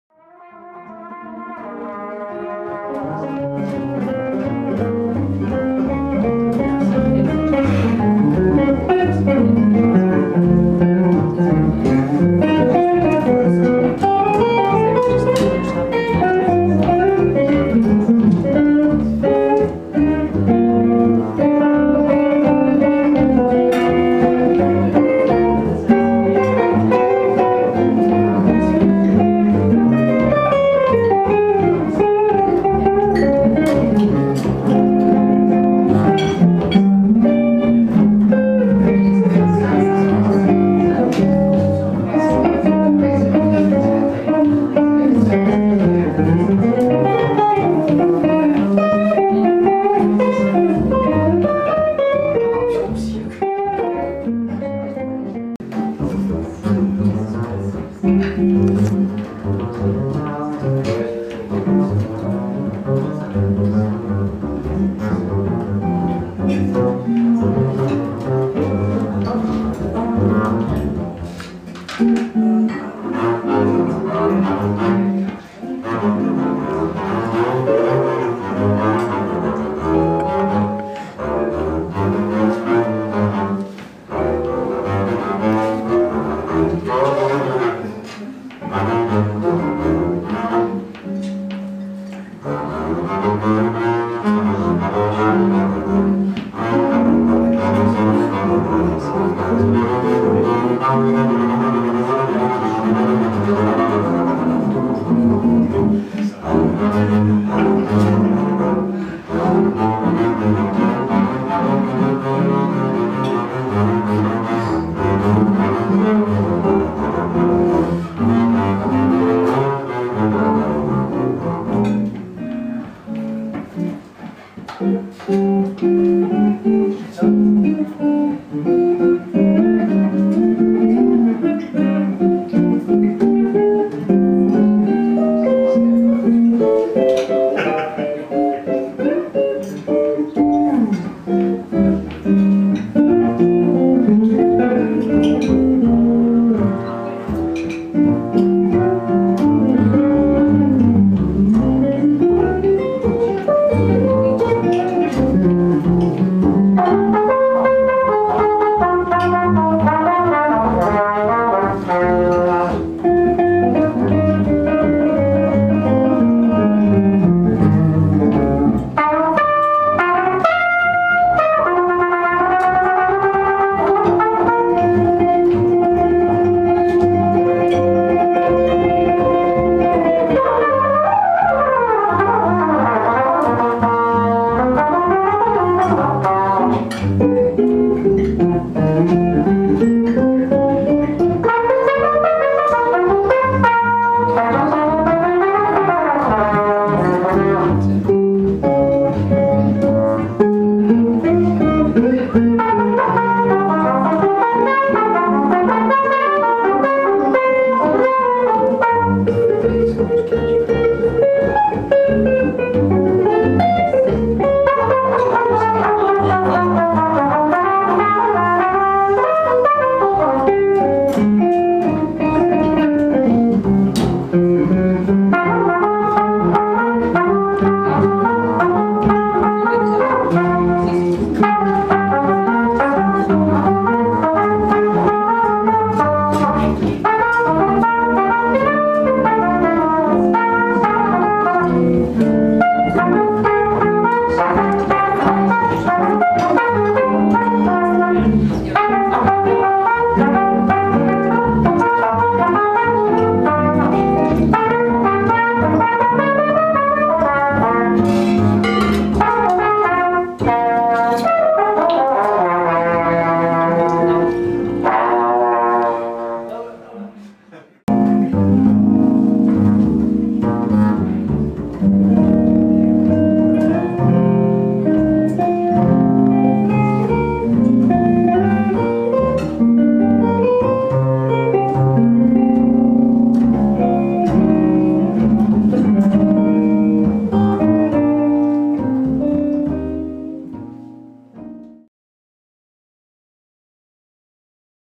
Porter, Bray & Young
John Porter (5-string double bass), Nathan Bray (trumpet) and Neil C. Young (guitar) playing at Stray's Coffee LAST THURSDAY JAZZ SESSIONS on May 29th.